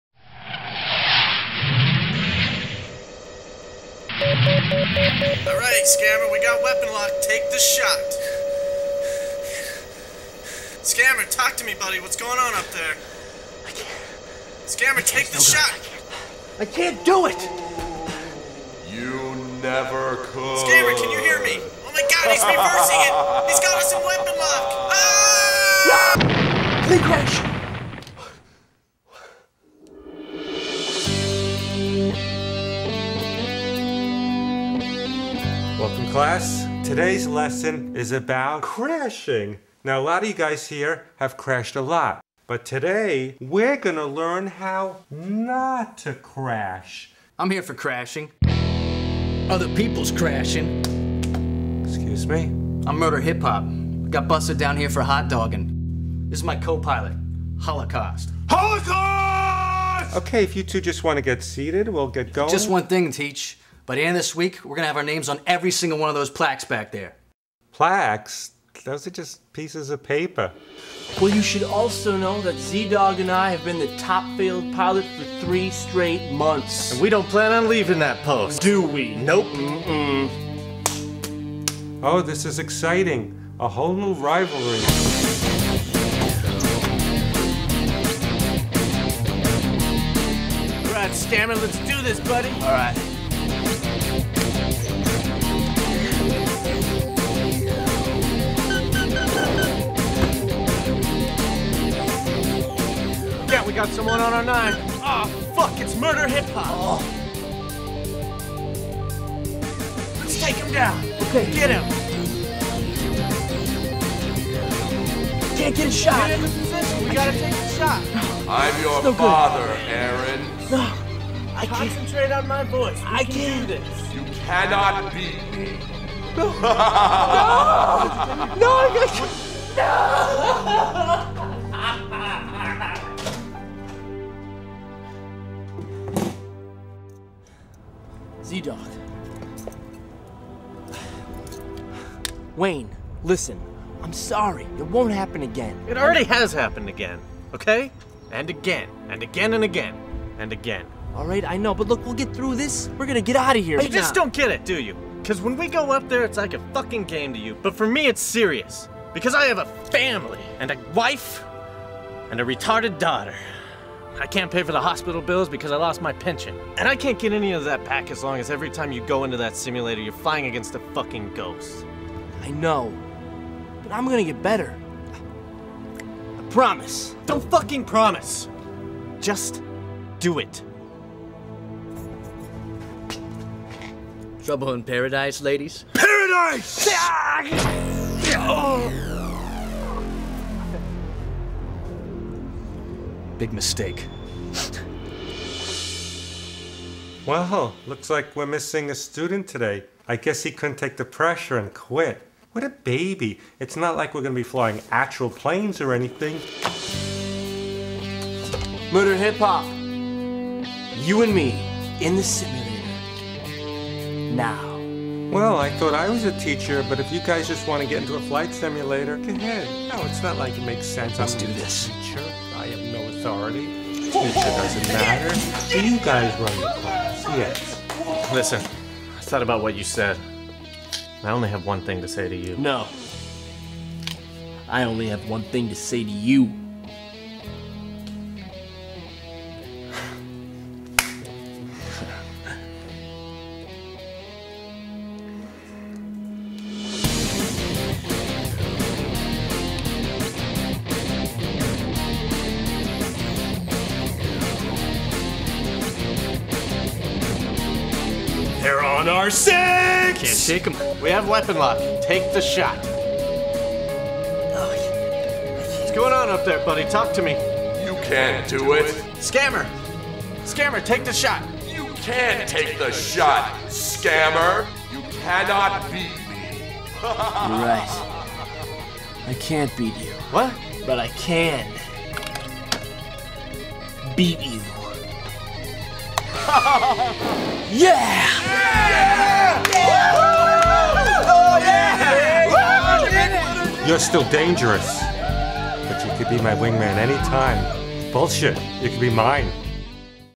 Top Failed Pilots
The worst fighter pilots in the world compete to be the best of the worst, the top failed pilot.